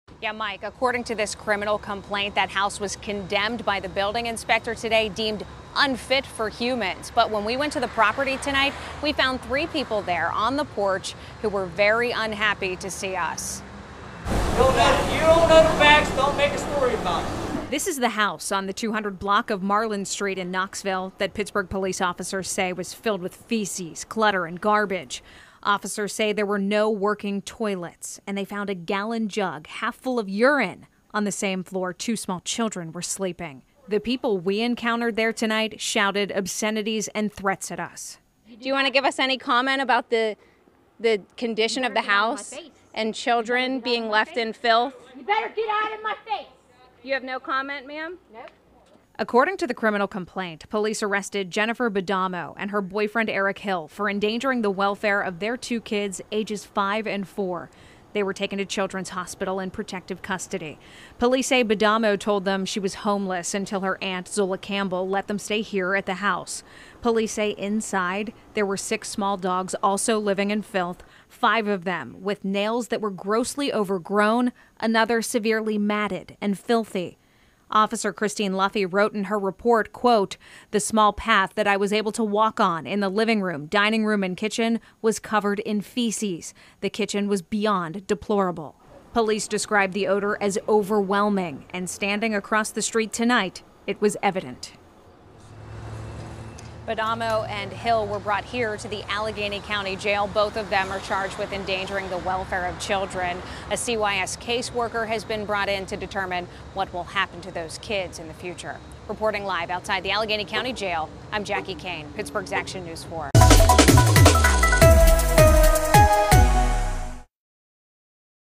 Two children removed from 'house of filth' in Pittsburgh's Knoxville neighborhood
Police found no working toilets, a jug with urine, and feces on the floor where two young children were sleeping